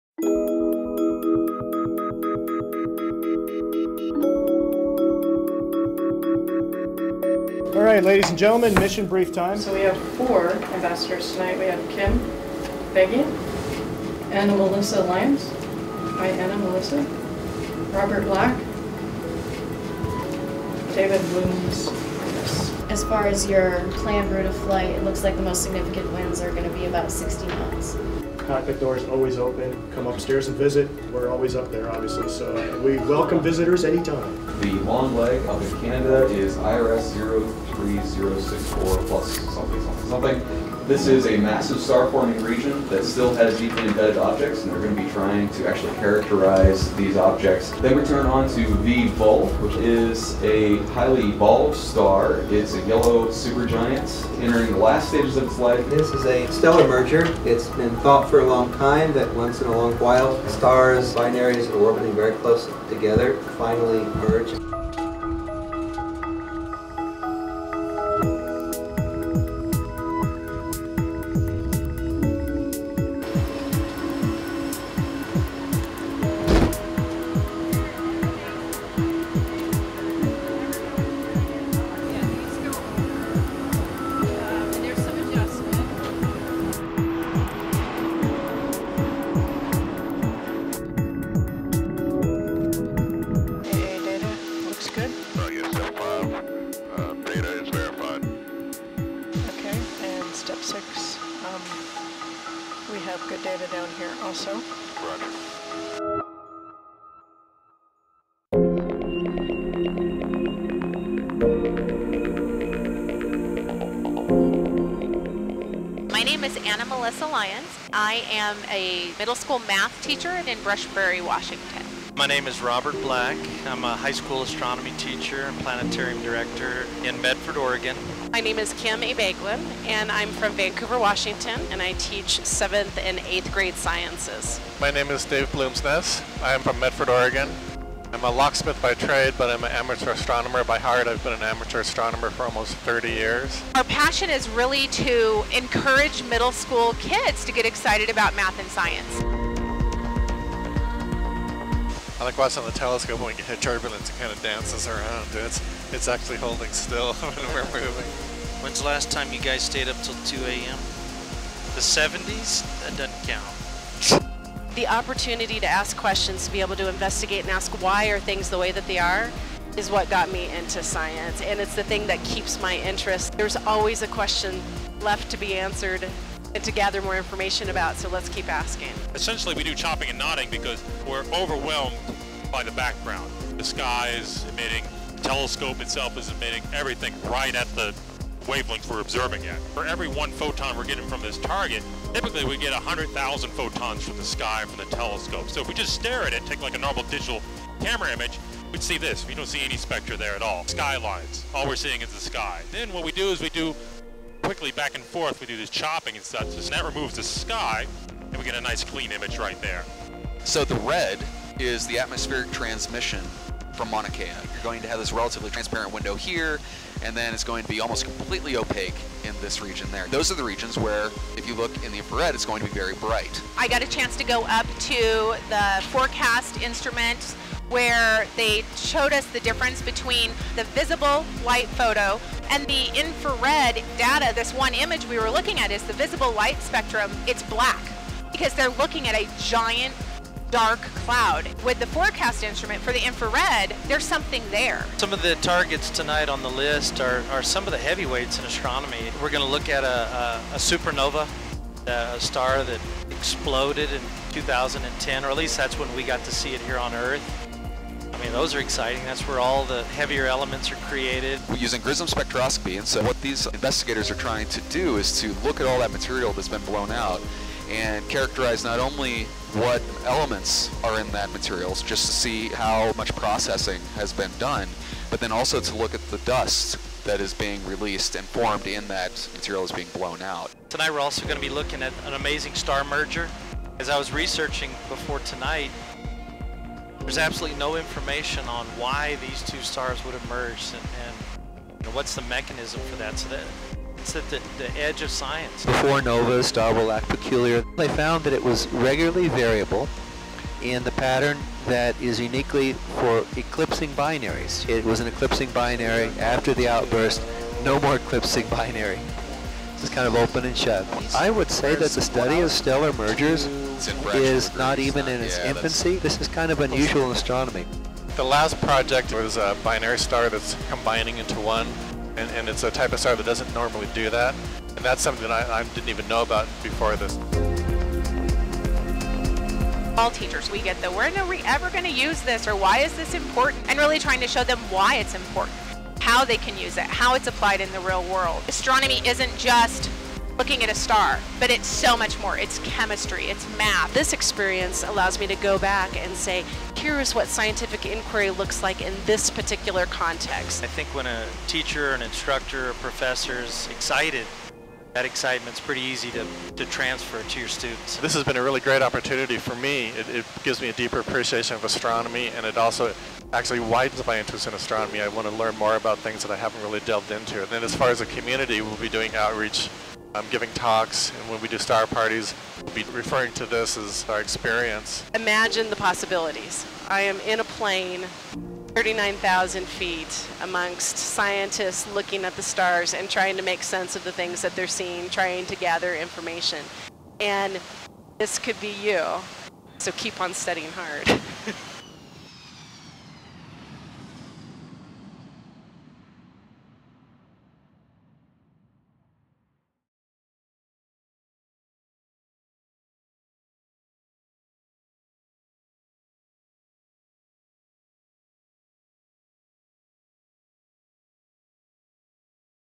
SOFIA Observatory Hosts Oregon, Washington Educators
Kim Abegglen and Anna-Melissa Lyons from Hockinson Middle School, Vancouver, Washington, Robert Black from North Medford High School and Dave Bloomsness from the Southern Oregon Skywatchers, Medford, Oregon, flew on board NASA's Stratospheric observatory for Infrared Astronomy (SOFIA) in May 2014. The educators observed SOFIA scientists studying star forming regions and a unique stellar merger during the flight. Follow along as they see infrared astronomy in action.

 In 2014, 12 two-person teams were competitively selected for SOFIA's Airborne Astronomy Ambassadors program, representing teachers from 10 states. After their flight opportunities, Airborne Astronomy Ambassadors will take what they learn back to their classrooms and into their communities to promote science literacy.